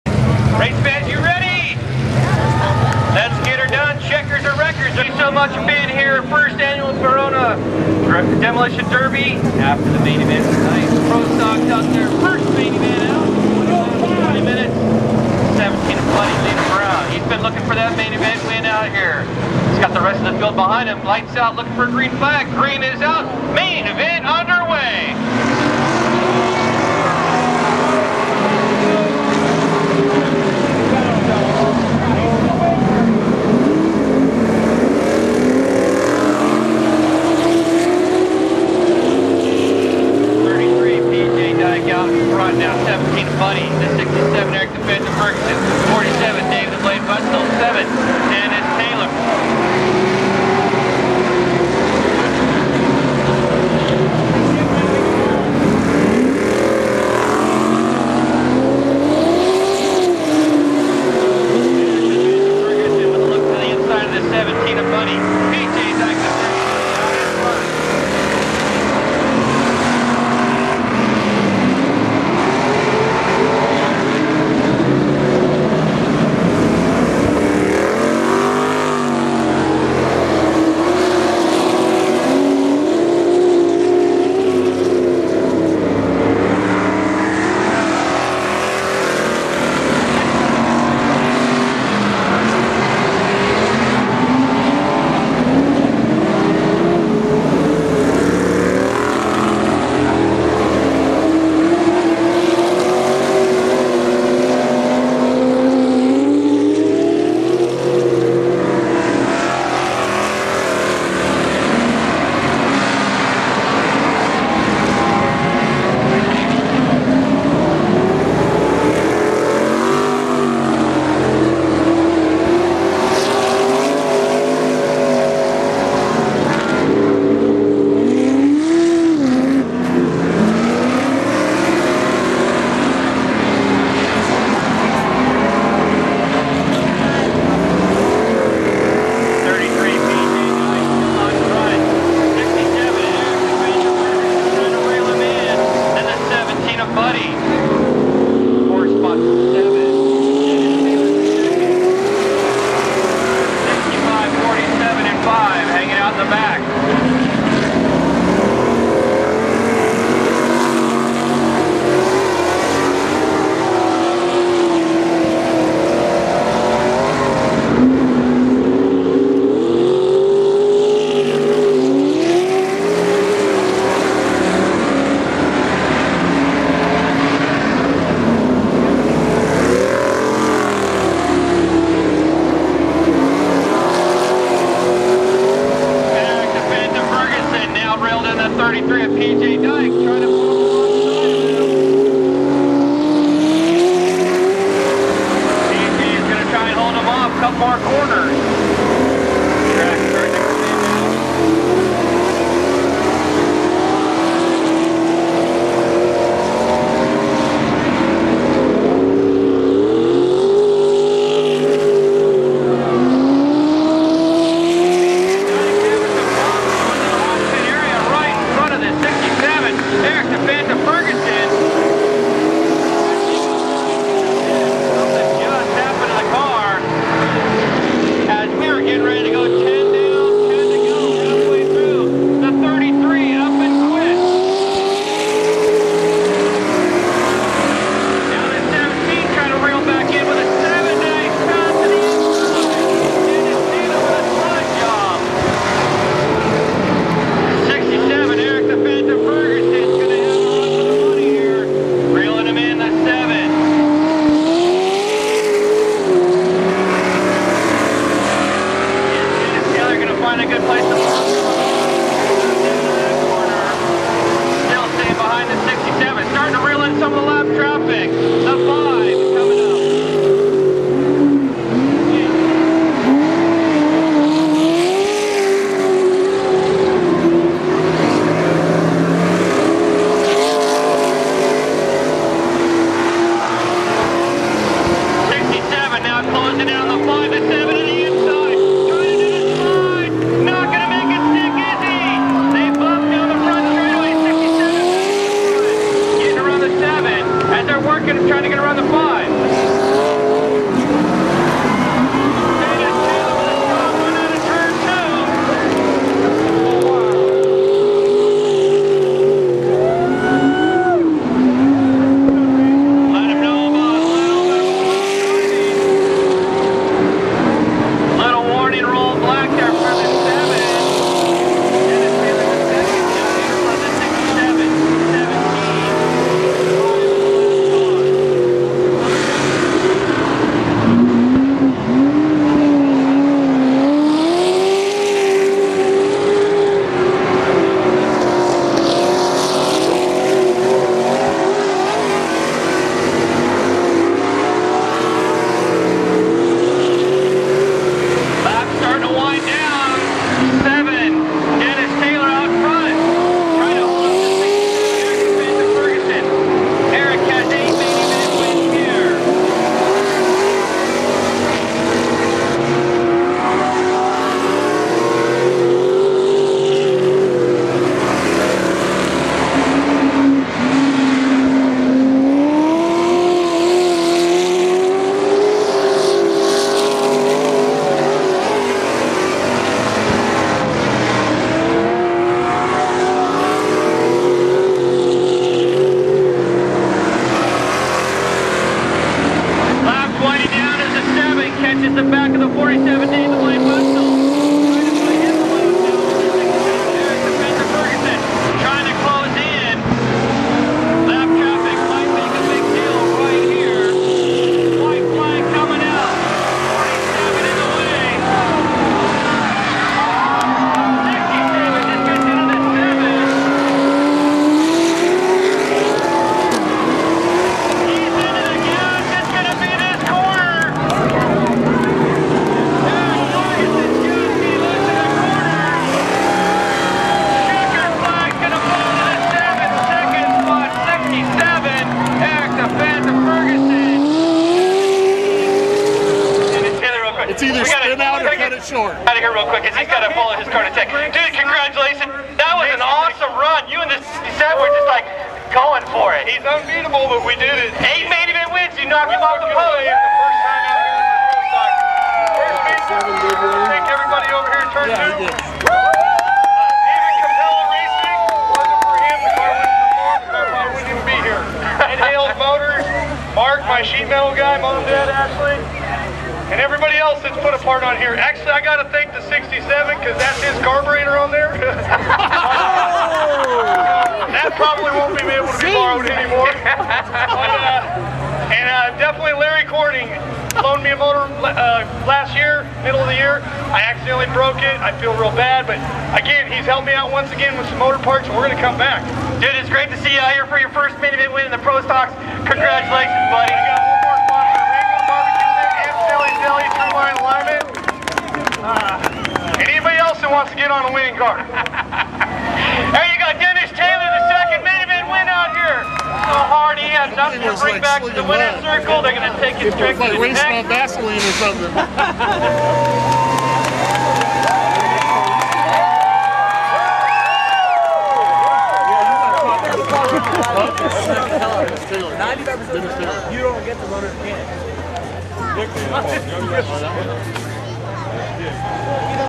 Pro Stock Main Barona Speedway 10-1-2011
First Main of the night with some hard racing from the #7 and #67 with the #7 dennis taylor winning with a broken rear end on the pro stock.